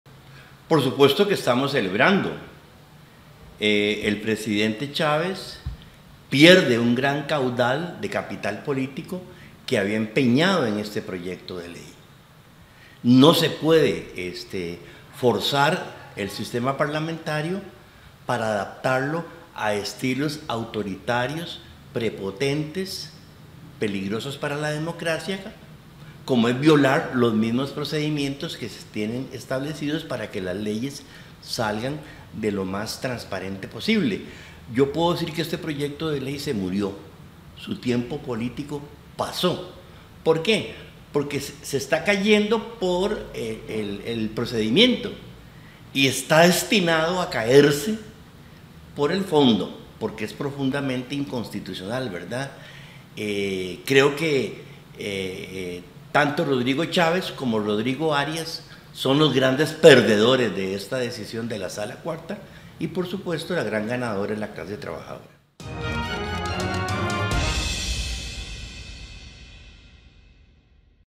Declaraciones de ANEP sobre lo determinado por la Sala Constitucional sobre las jornadas 4x3
Actualidad de Costa Rica y el mundo.